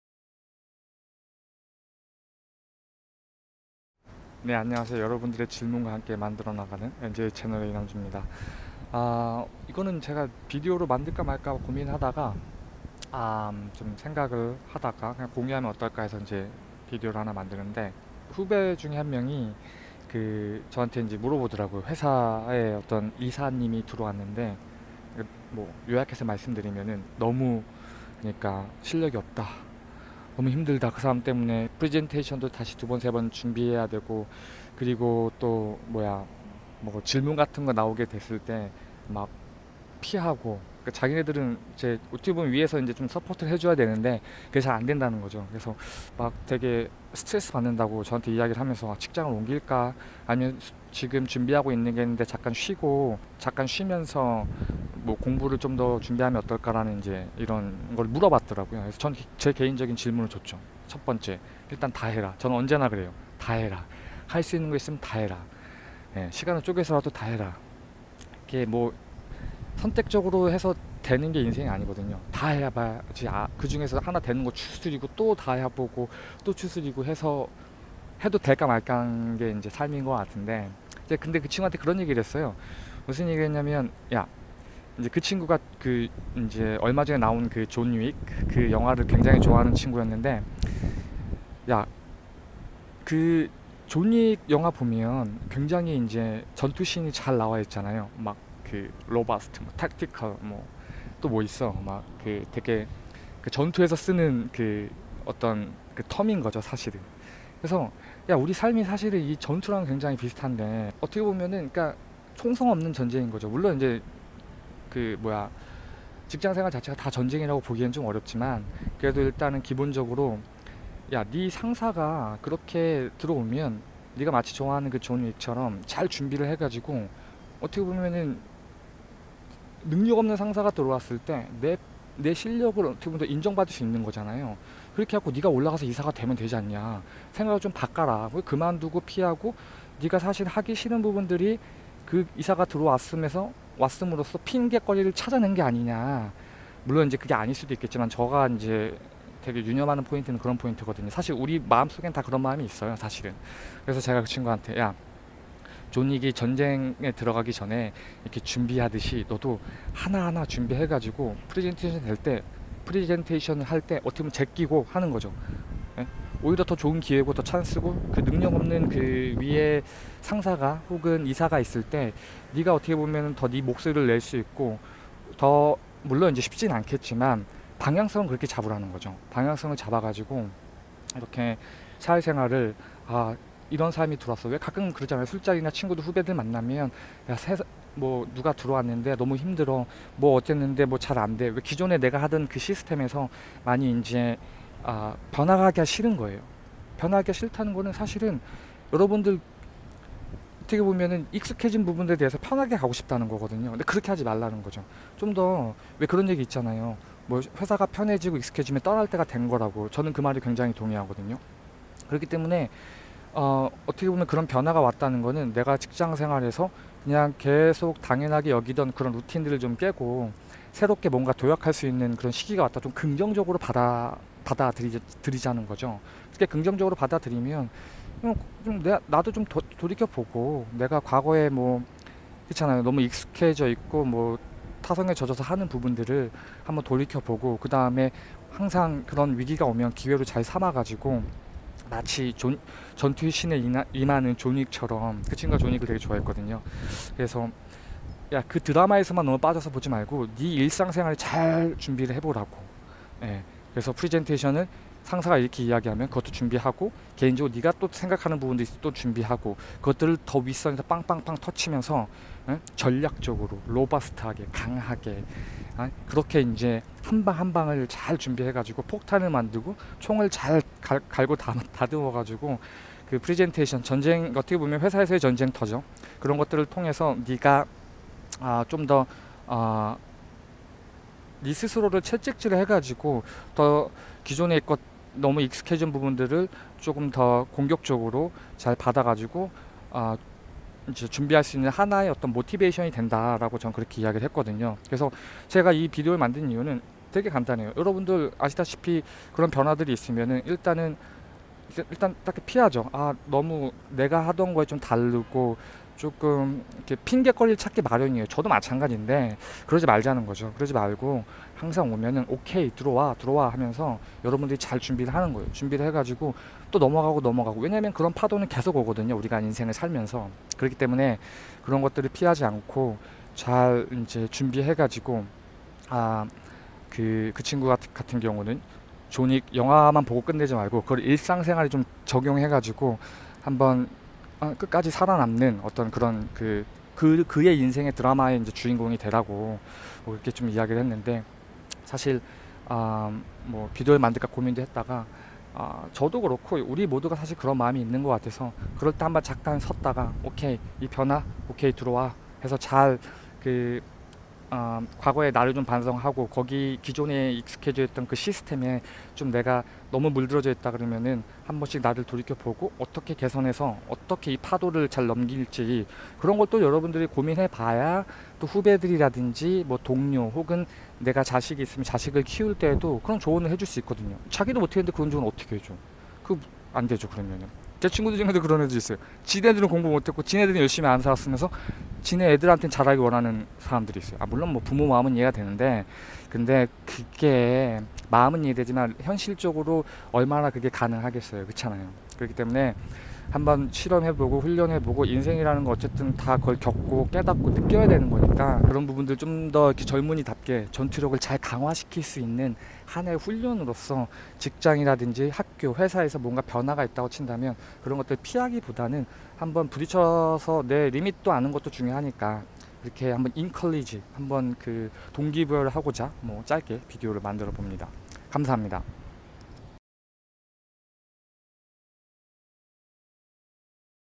NJC_QnA 09, 직장상사가 내 미래에 도움이 안될 때, 회사를 떠나야하나? 존윅에서 배울점
존윅 영화를 좋아하는 후배와 나눈 대화입니다. 직장상사가 내 미래에 도움이 안 될 때… 회사를 옮겨야할까요? 존윅 같이 전투력을 기릅시다!